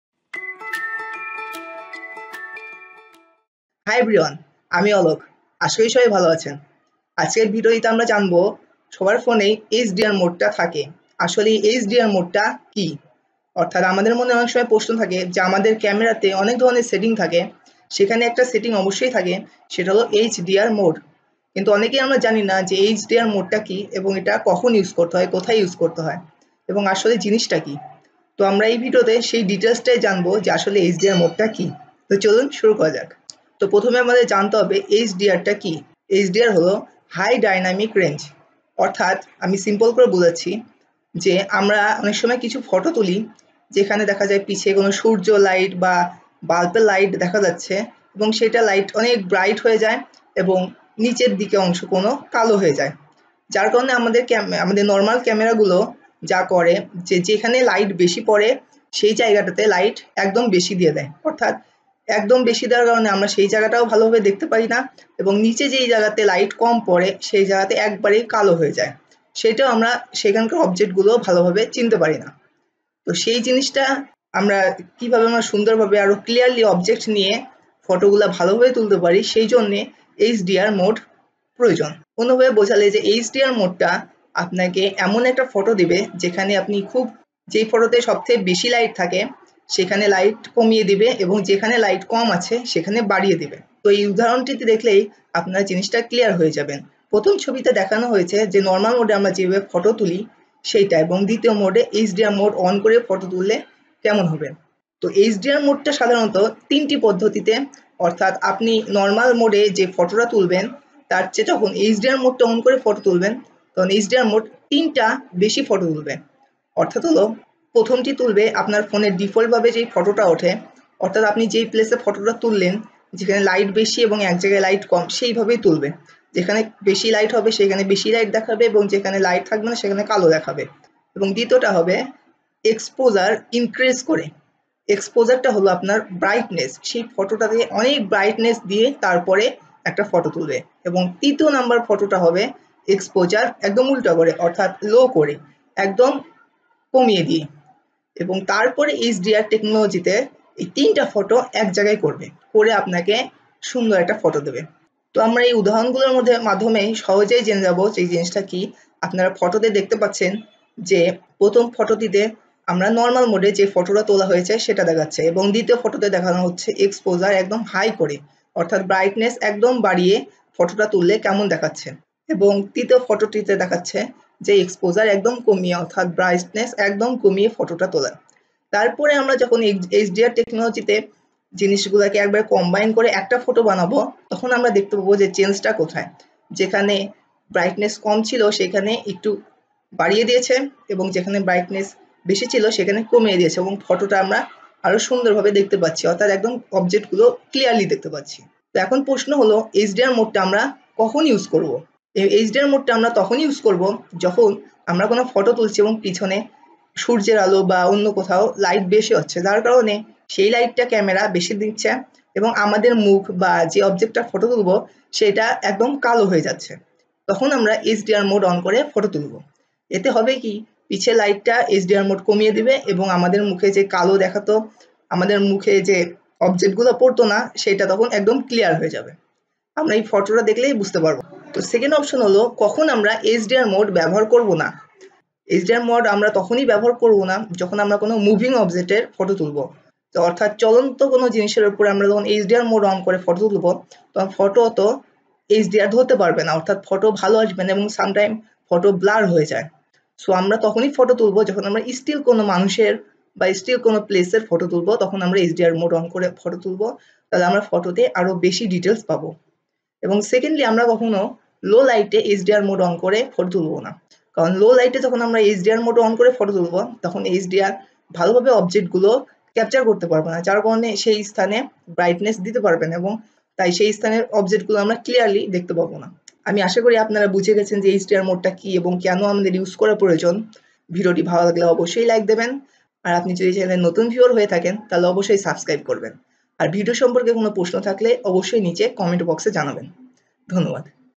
What is HDR in Camera? How it works? How useful it is? - Explained in Bangla | Aroundthealok
Bondhura, in this video you'll know What is HDR in Camera? How it works? How useful it is? - Explained in Bangla. HDR technology means High Dynamic Range. You'll also know What is an HDR photo or image on iPhone on any phone, what is high dynamic range, how HDR camera works, what is HDR+, what is HDR photography and when we should use HDR. Watch this video fully to know. I hope you'll like this video.

About: Aroundthealok is a YouTube Channel, where you will find technological videos in Bangla, New Video is Posted on Every week. :)